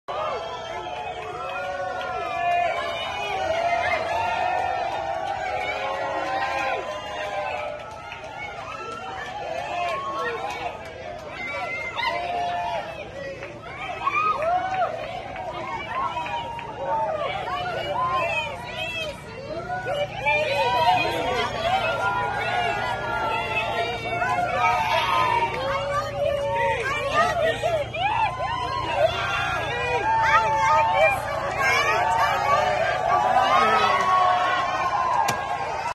Keith Richards and Ronnie Wood leaving the hotel on their way to the show at the Waldbühne, Berlin
Keith and Ronnie are ready to go to the Waldbühne to do the last gig of the SIXTY tour.

source: officialkeef IG